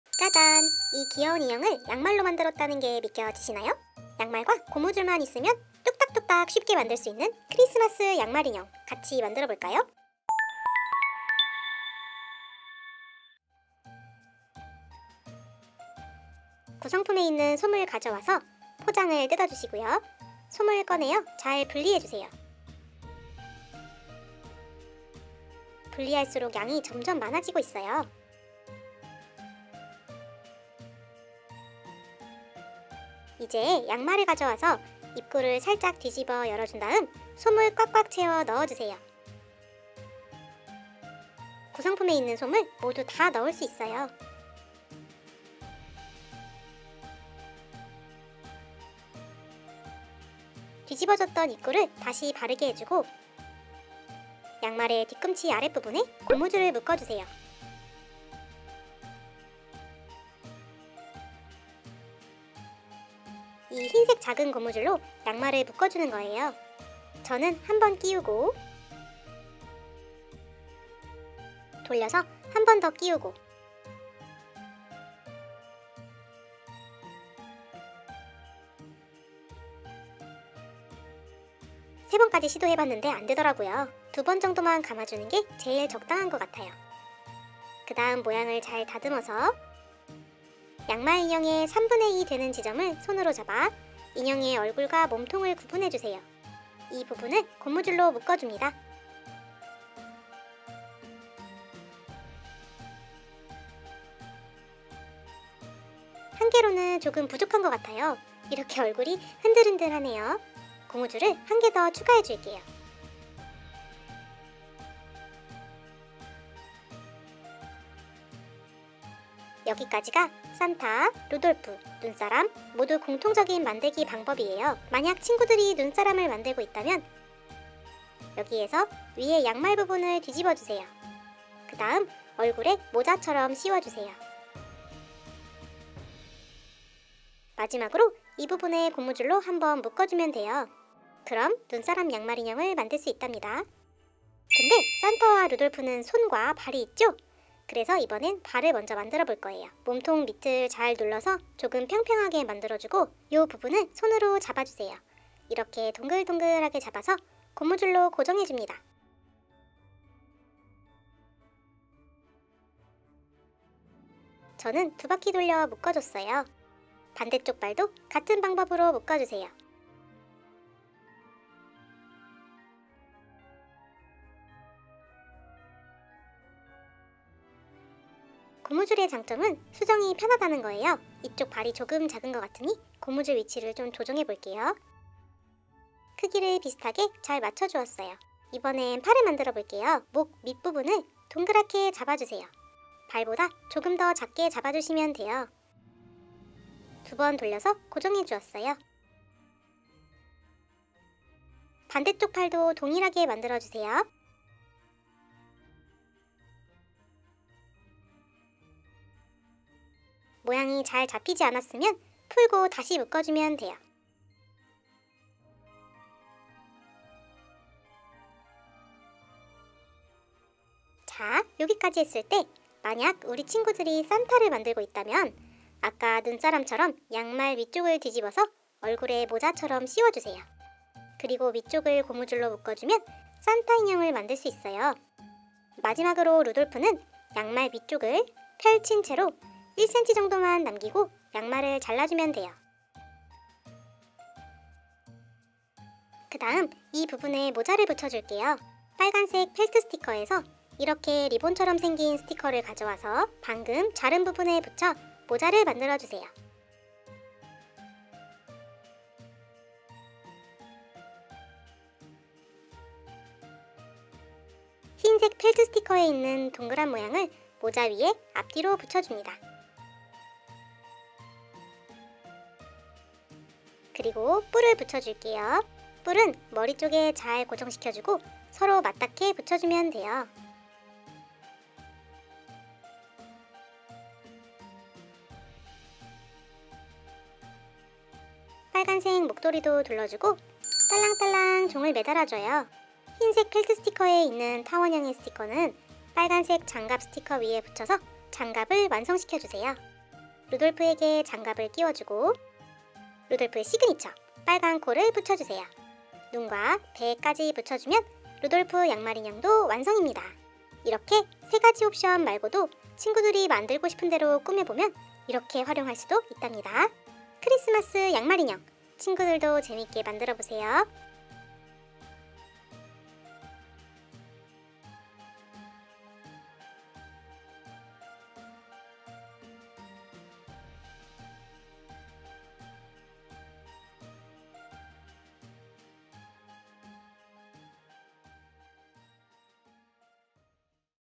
바느질 없이 크리스마스 양말인형 만들기 I 크리스마스 만들기, 양말인형, 산타할아버지, 겨울만들기, 펠트양말인형, 미술재료, 간단한만들기, 쉬운만들기
어려운 바느질 없이 양말 인형을 만들 수 있다구?
양말과 고무줄만 있으면 뚝딱 만들어지는 양말인형이에요!
크리스마스 시즌을 맞아
눈사람/산타할아버지/루돌프 3가지 만드는 방법을
모두 모두 알려드릴게요 😍 !